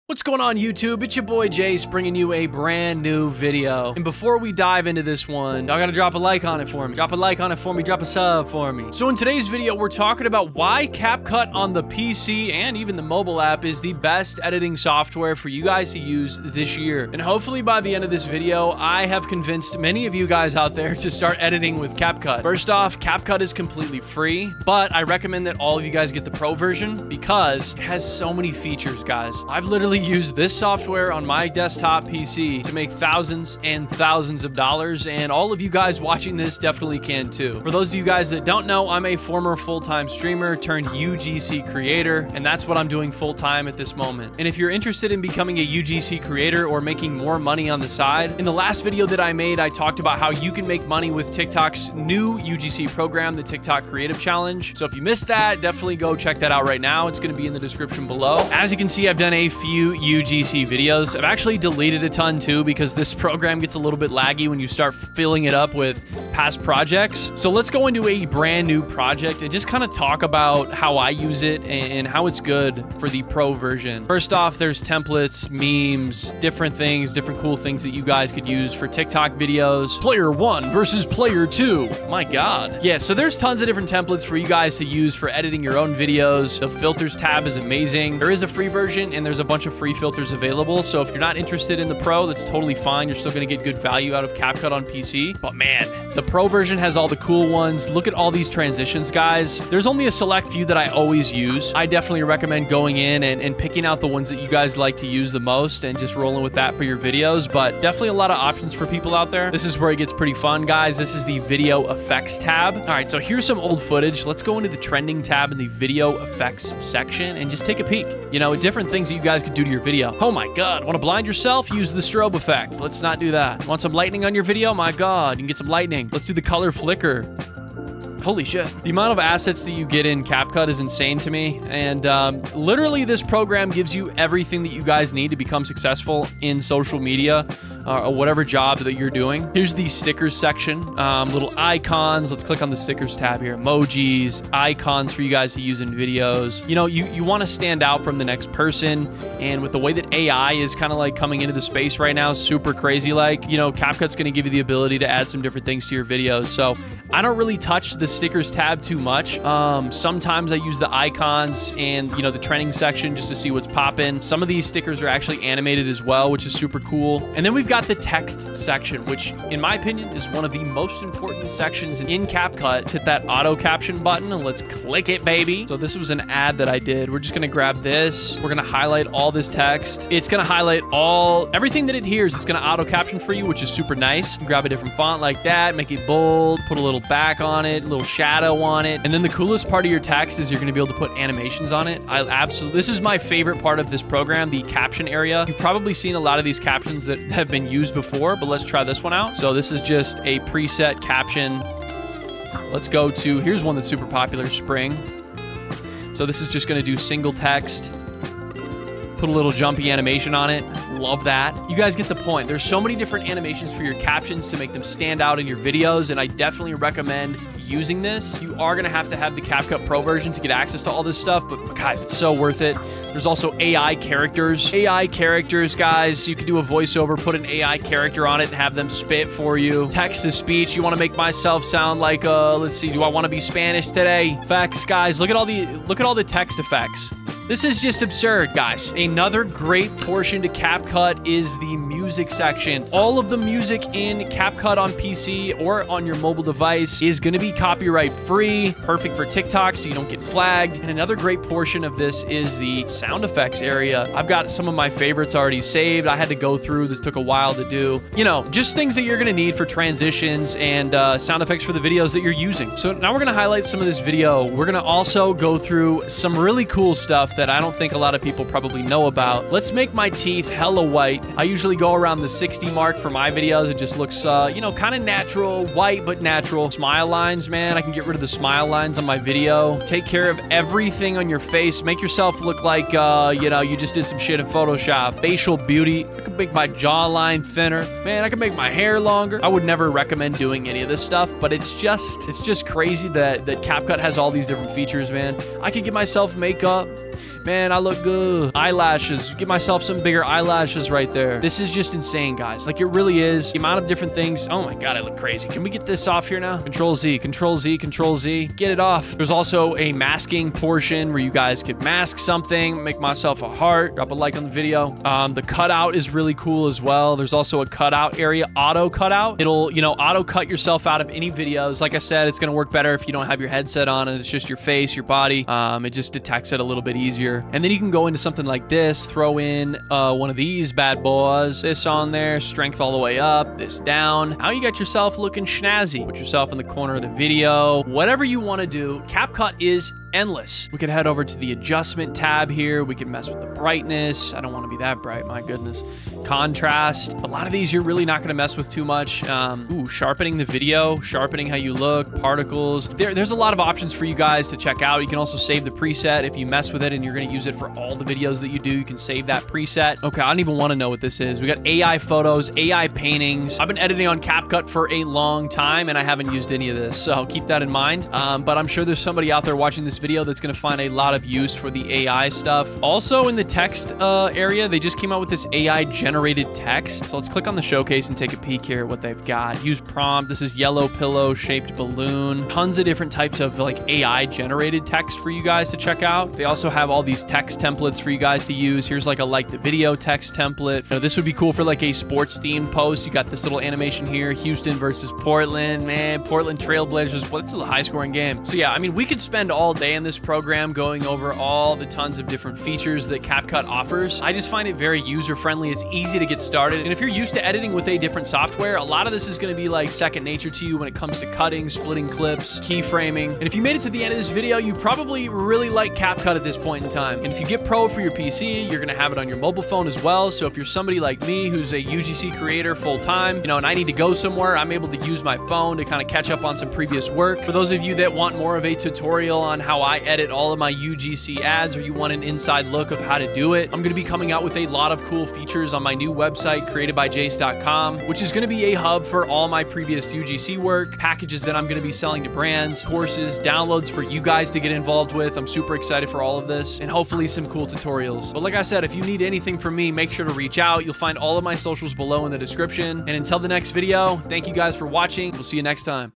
Capcut for PC is the Best Editing Software for all Creators in 2024!
In this video, I discuss the reasons why Capcut and its PC version stand out as the optimal choice for video editing software in 2024, catering to both beginners and advanced creators. I explore the software's capabilities by showcasing its impressive features and explaining the advantages of using it. Capcut boasts exceptional sound effects, automatic captioning, copyright-free music, seamless transitions, and a host of other functionalities. Additionally, I delve into the enhanced features offered by the pro version of Capcut for PC.

0:00 : INTRO
1:00 : Capcut Features
6:45 : Outro